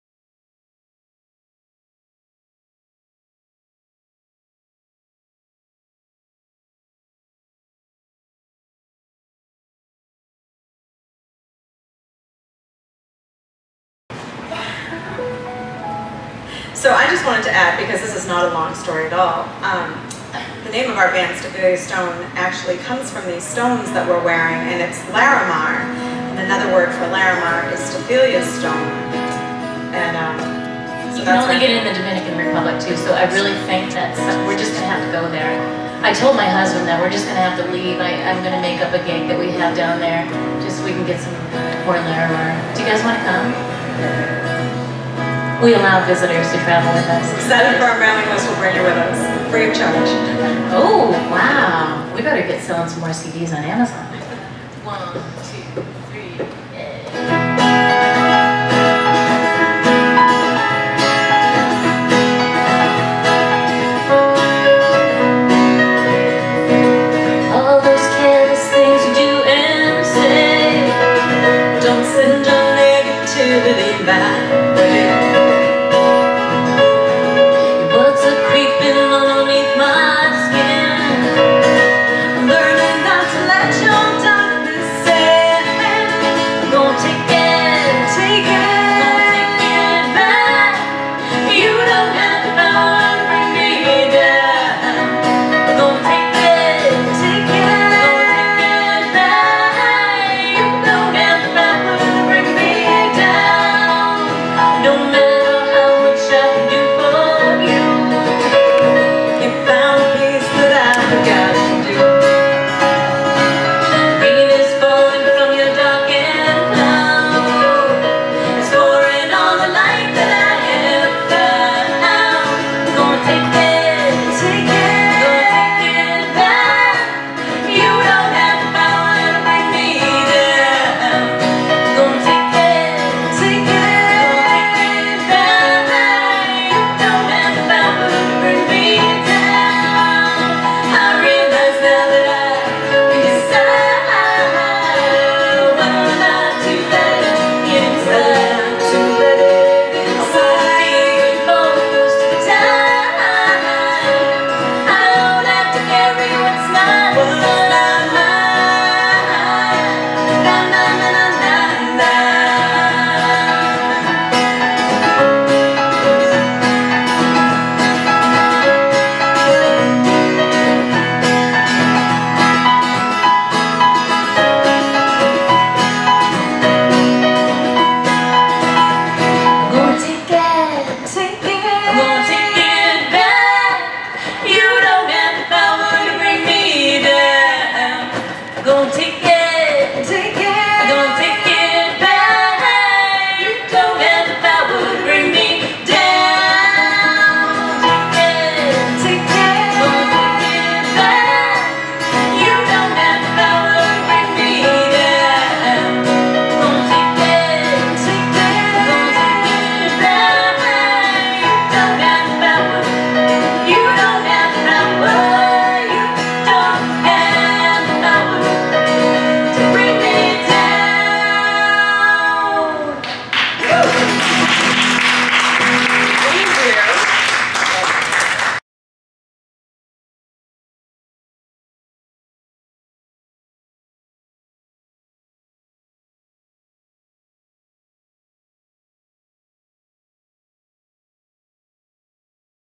Stefilia's Stone at 2016 Franklin Cultural Festival
Here is a sample of the performance by Stefilia's Stone at THE BLACK BOX under the tent on the outdoor stage presented by the Circle of Friends Coffeehouse.